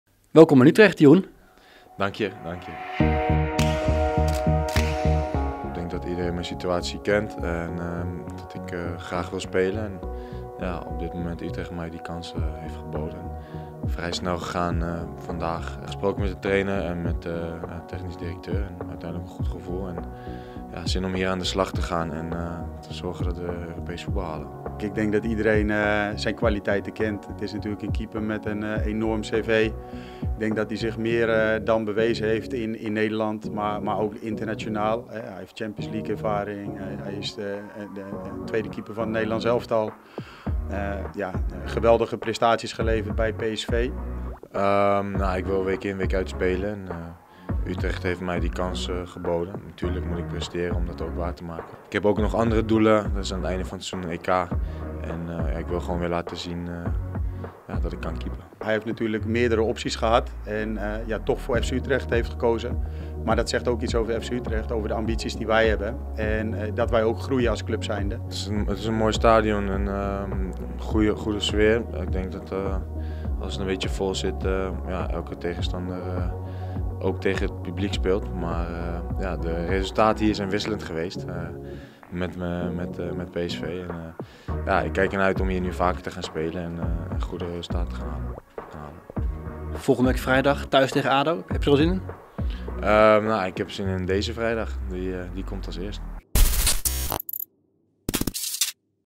ZOET | 'Zin om aan de slag te gaan'
FC Utrecht heeft zich per direct versterkt met Jeroen Zoet. De 29-jarige doelman wordt door de Domstedelingen voor de rest van het seizoen gehuurd van PSV.